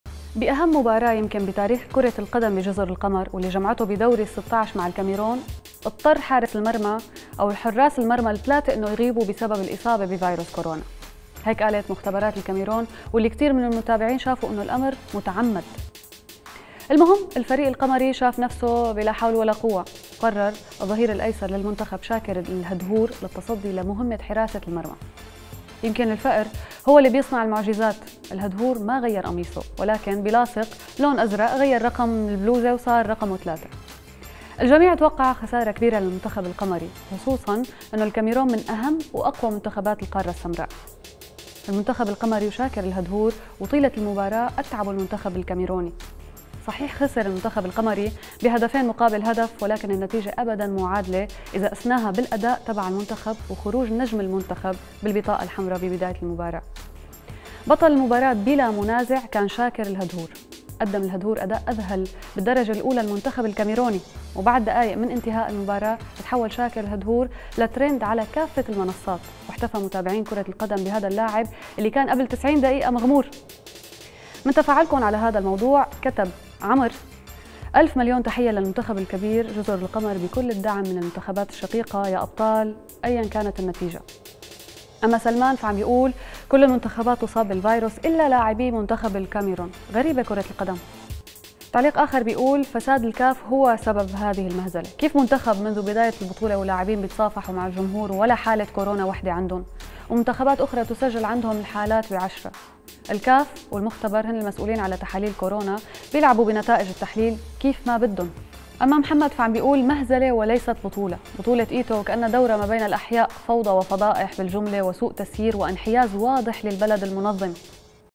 مدافع فريق جزر القمر يأخذ دور حارس المرمى بسبب كورونا
مدافع فريق جزر_القمر يأخذ دور حارس المرمى بسبب
لمتابعة الحلقة كاملة: